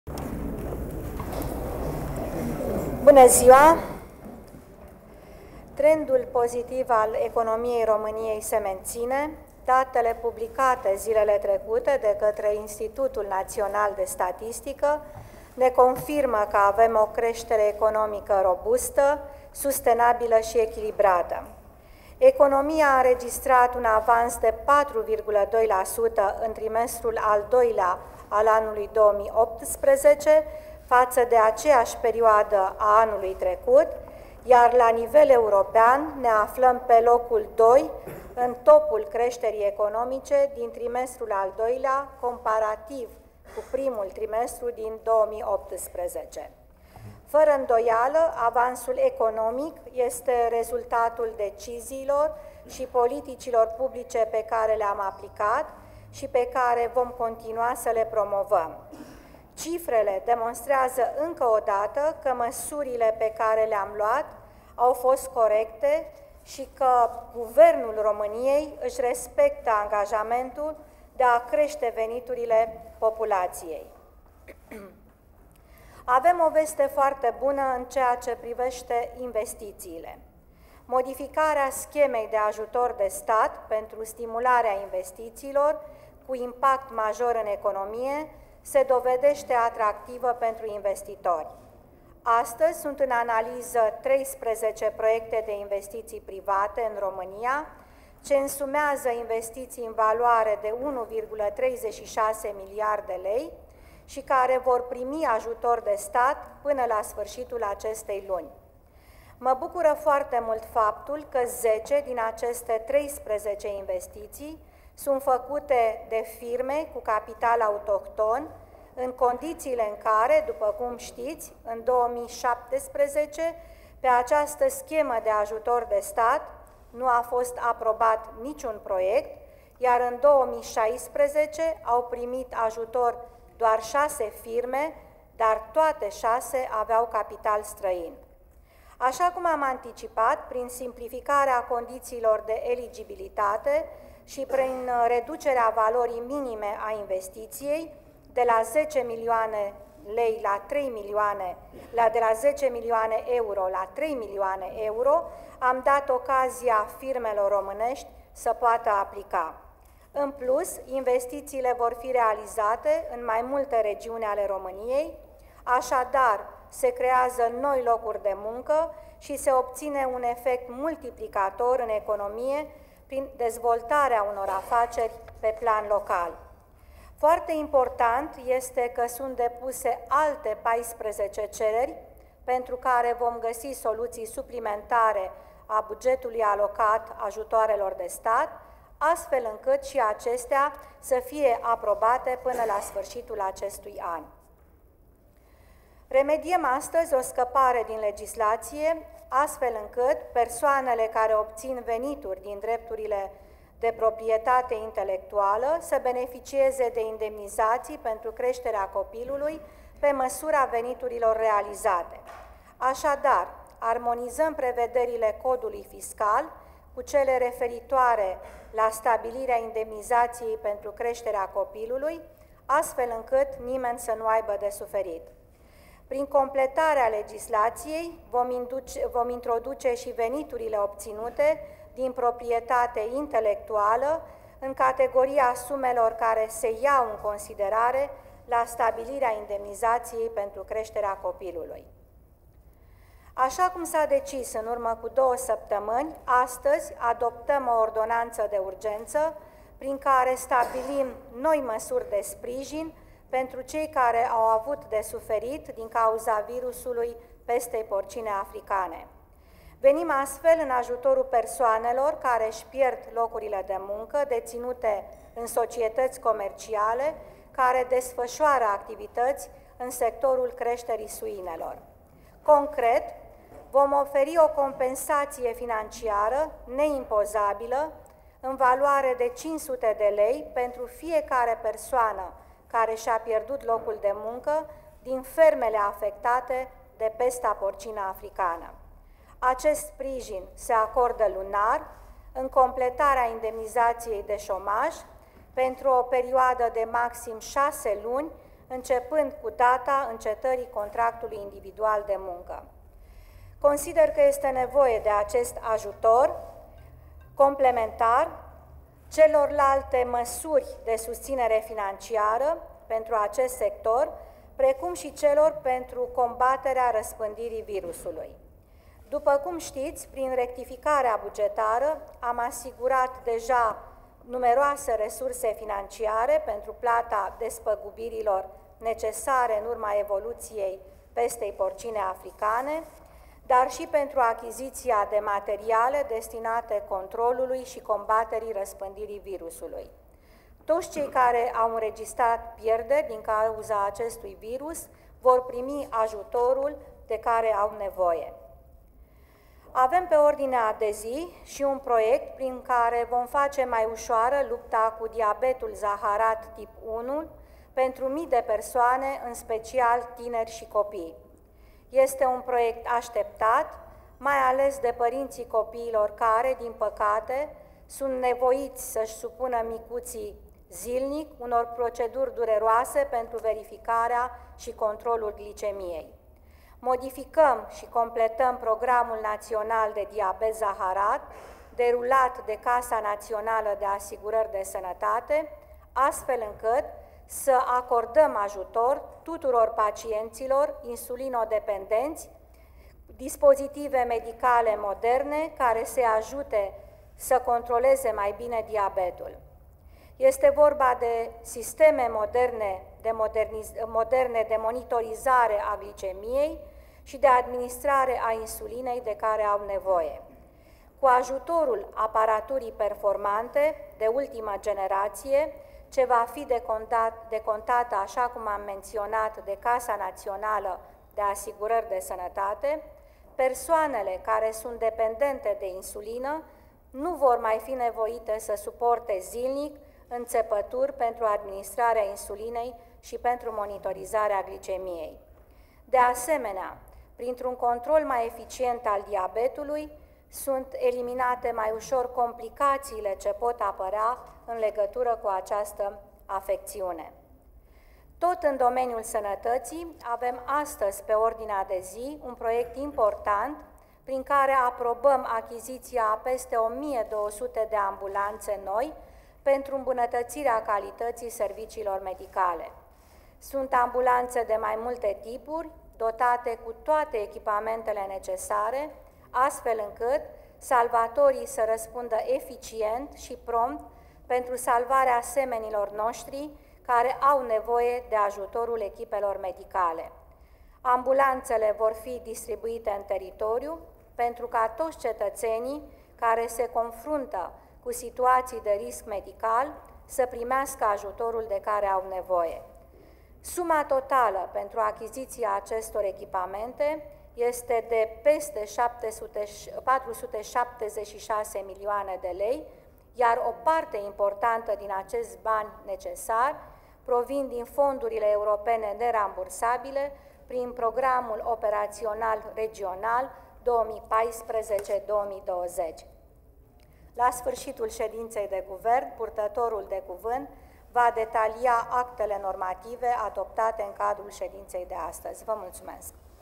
9/13/18 Declarații susținute de premierul Viorica Dăncilă la începutul ședinței de guvern
9/13/18 Palatul Victoria: Declarații susținute de premierul Viorica Dăncilă la începutul ședinței de guvern